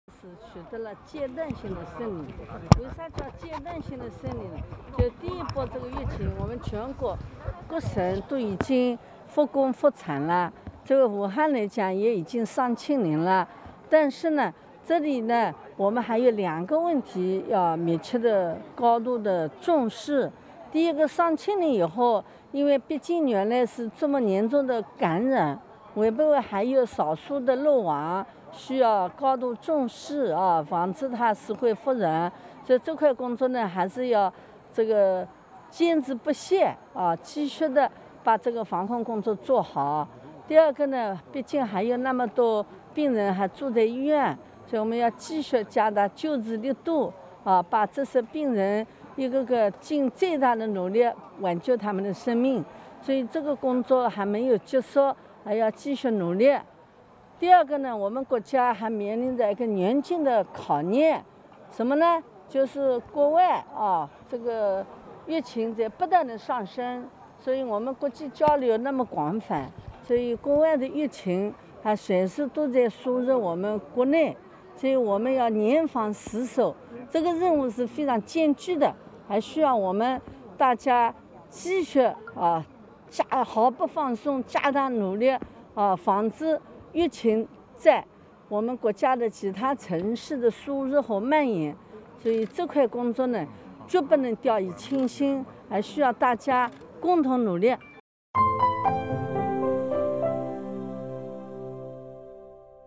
武汉新增病例“归零” 李兰娟称当前两个问题要高度重视
3月23日，中国工程院院士、国家卫健委高级别专家组成员李兰娟在武汉大学人民医院东院接受采访时表示，全国不少省区市已复工复产，武汉也已经多日报告新增新冠肺炎确诊及疑似病例“归零”。但当前依然有两个问题值得高度重视。

一是社会上可能还有“漏网”病例，需高度重视防止疫情“死灰复燃”，这块工作要坚持不懈；当前病患存量还较高，应继续加大救治力度，尽最大力度挽救重症、危重症患者生命。二是中国正面临严峻的境外输入病例考验，需严防死守，对此决不能掉以轻心。（郑子颜）